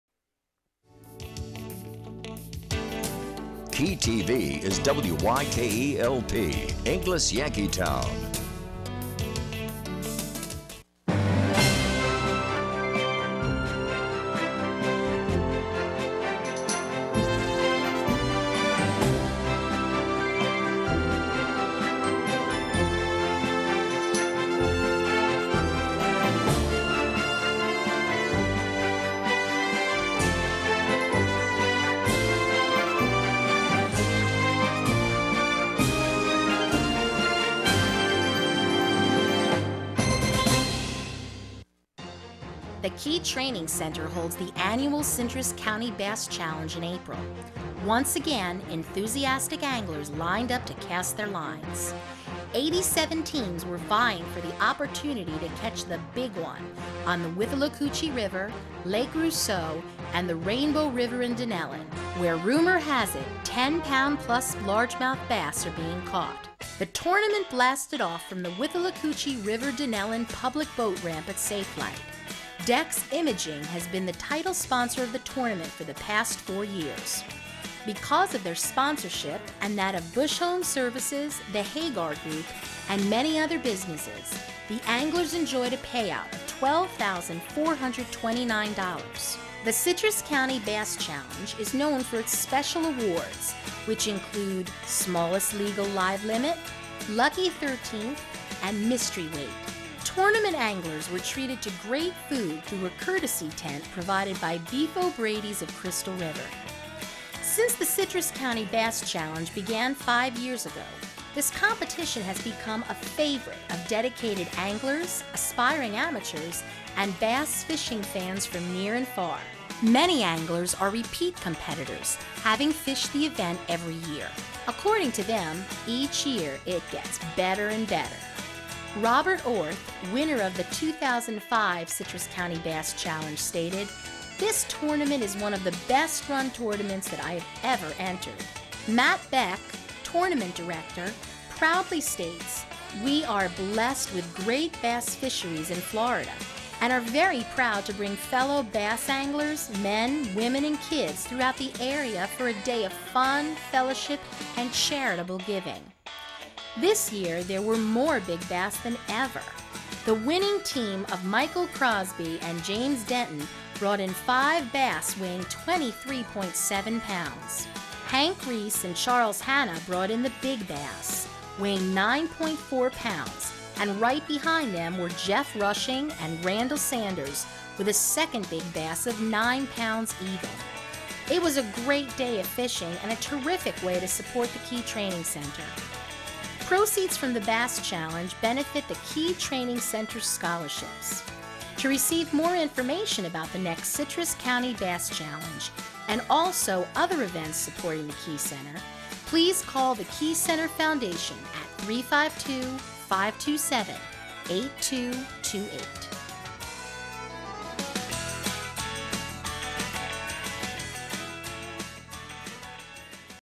Bass Challenge 2009
An overview of the 2009 Citrus County Bass Challenge for the Key Training Center.